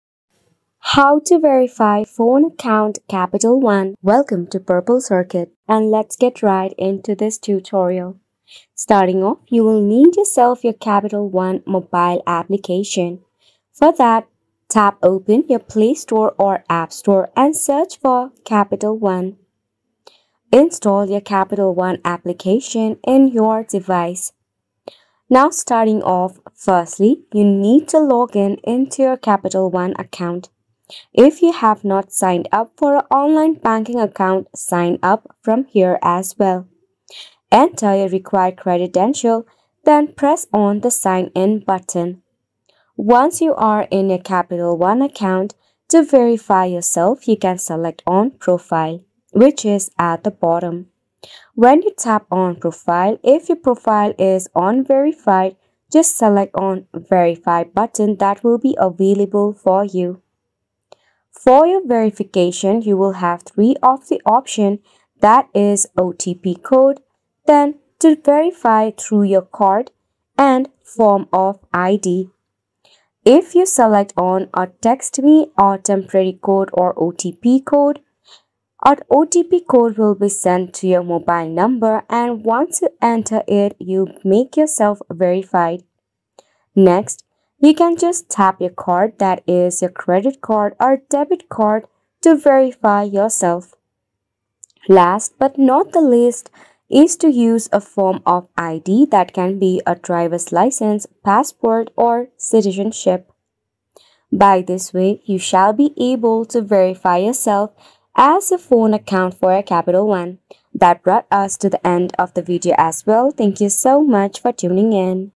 How to Verify Phone Account on Capital One Online (2024)
This guide will show you how to verify your phone account on Capital One Online. Our step-by-step instructions will make the verification process smooth and simple, ensuring your account is secure and fully functional.

In this video, you’ll learn:

- How to access your Capital One account online
- Navigating to the phone verification section
- Steps to verify your phone number
- Ensuring your account security

Capital One makes it easy to verify your phone account online. Our guide covers everything you need to know to complete this process efficiently.

Make sure you stay till the end for some tips on maintaining account security and troubleshooting common issues.